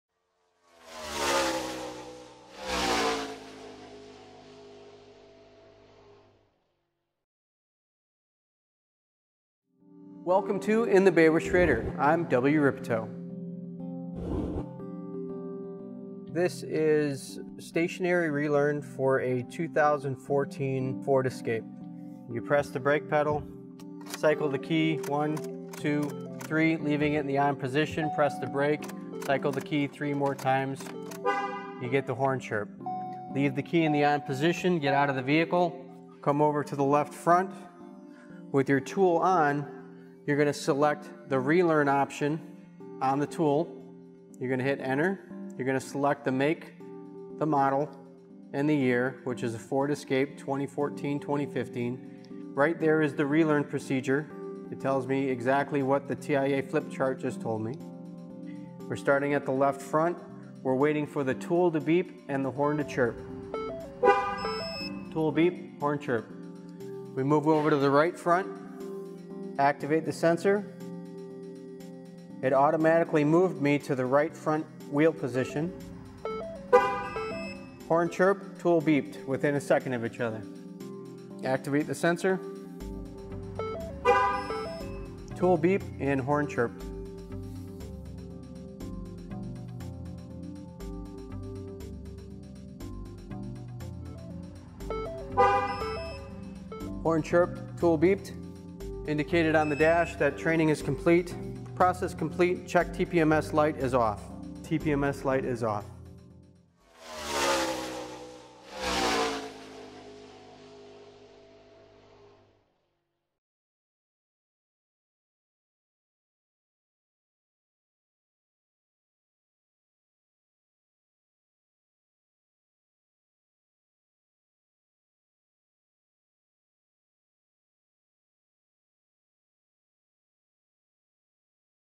TPMS Stationary Relearn 2014 Ford Escape | Know Your Parts
Connect with Know Your Parts to learn more.

Transcript:
Welcome to in the bay by Schrader. I'm W. Ripto. This is stationary relearn for a 2014 Ford Escape. You press the brake pedal. Cycle the key 1, 2, 3, leaving it in the on position. Press the brake. Cycle the key 3 more times. You get the horn chirp. Leave the key in the on position. Get out of the vehicle. Come over to the left front with your tool on. You're going to select the relearn option on the tool. You're gonna hit enter. You're gonna select the make, the model, and the year; which is a Ford Escape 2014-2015. Right there is the relearn procedure. It tells me exactly what the TIA flipchart just told me. We're starting at the left front. We're waiting for the tool to beep and the horn to chirp. Tool beep. Horn chirp. We move over to the right front. Activate the sensor. It automatically moved me to the right front wheel position. Horn chirp. Tool beeped. Within a second of each other. Activate the sensor tool. Beep and horn chirp. Horn chirp. Tool beeped. Indicated on the dash that training is complete. Process complete. Check TPMS light is off. TPMS light is off.